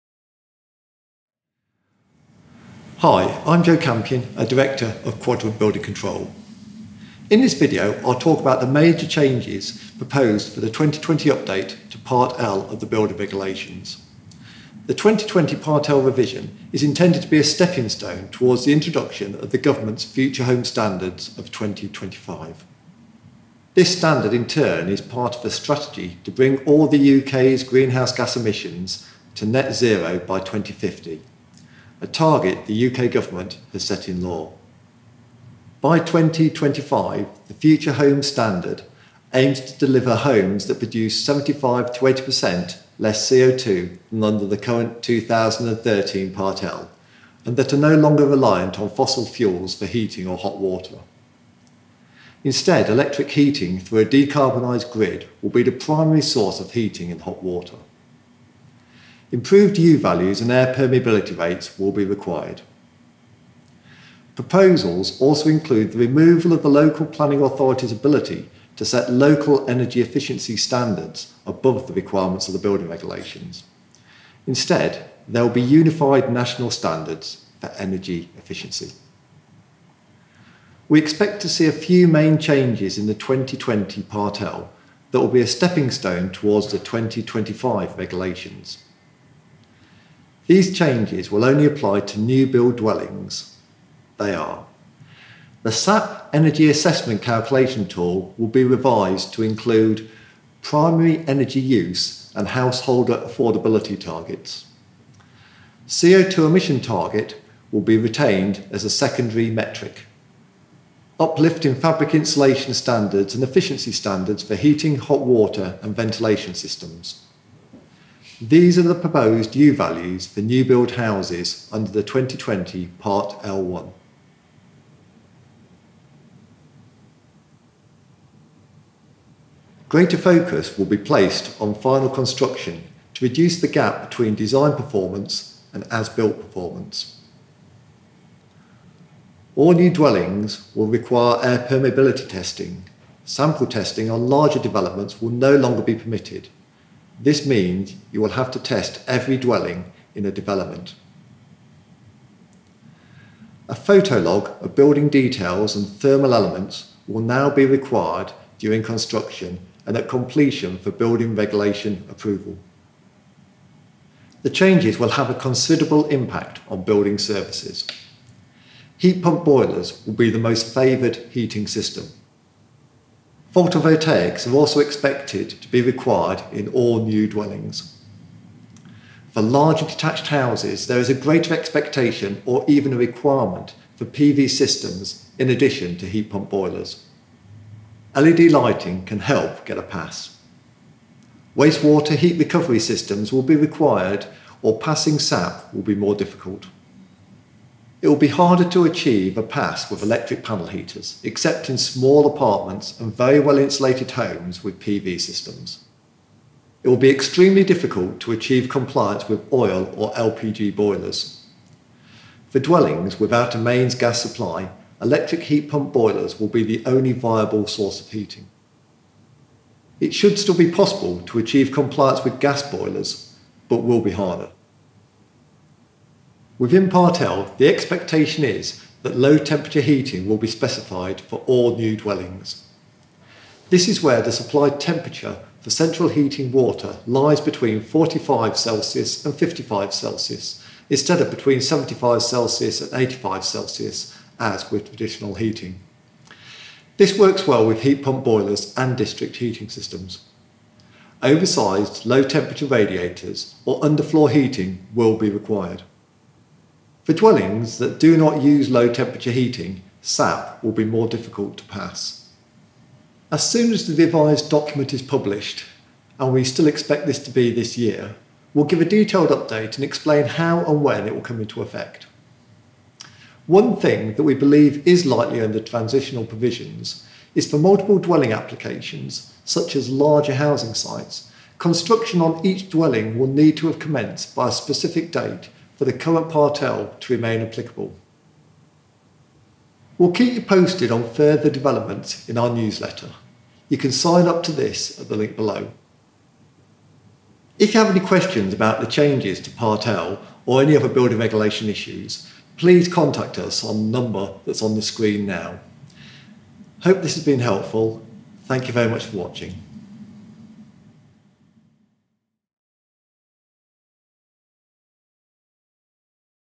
2020 UPDATE TO PART L – What it means for new housing
Joe Campion from Quadrant Building Control talks us through the likely big changes for dwellings in the new 2020 Part L.

The information contained in this video and in the accompanying article is believed to be correct at the present time, but is intended as advice only. Always consult with your building control body before undertaking construction work. November 2020.